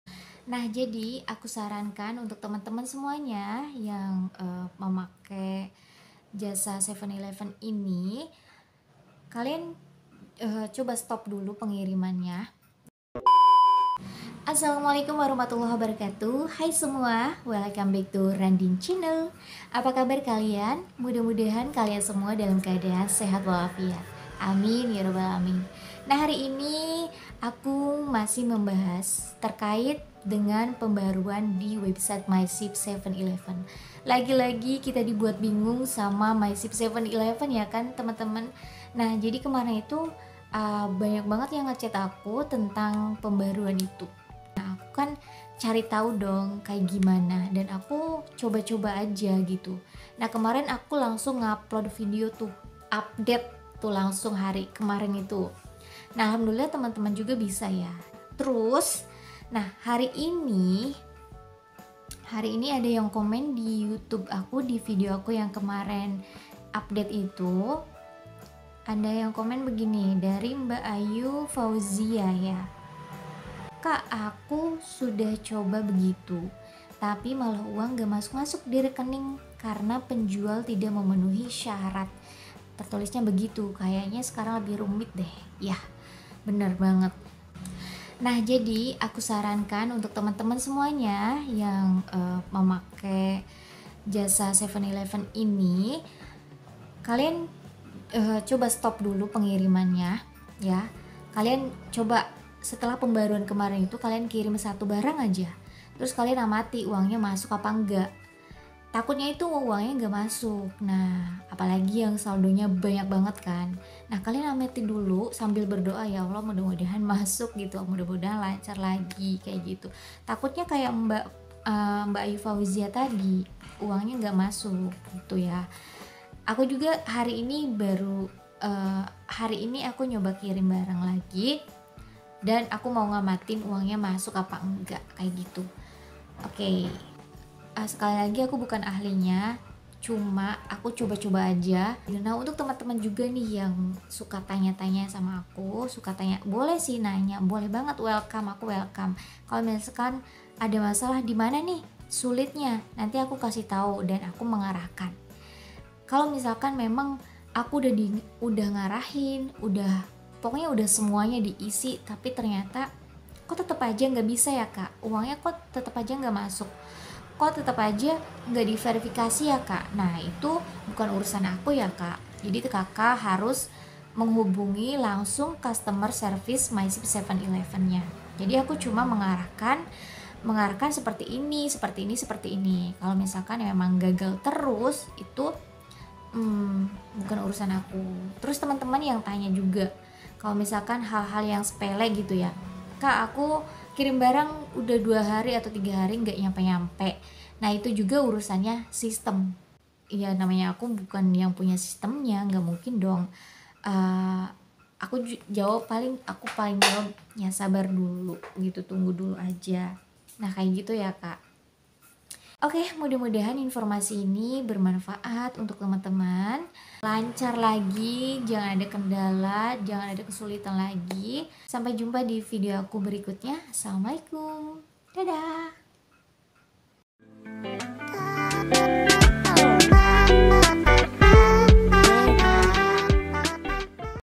STOP JANGAN KIRIM BARANG DULU❗PENJUAL TIDAK MEMENUHI SYARAT🤔⁉️
Stop jangan kirim barang dulu❗,
Lagi-lagi dibuat galau sama myship 7-11,

Setelah pembaharuan kemarin,,ada salah satu teman yg uang nya tidak masuk ke rekening alias gagal,
Alasanya penjual tidak memenuhi syarat,
Aku saranin ke temen2 yg lain,
Jangan kirim banyak2 dulu,mencoba dulu satu barang,lalu amati uang nya masuk atau tidak,
Kalau masuk alhamdulillah,
Tapi yg di takutkan kasus nya sama seperti temen kita yg gagal masuk uang nya,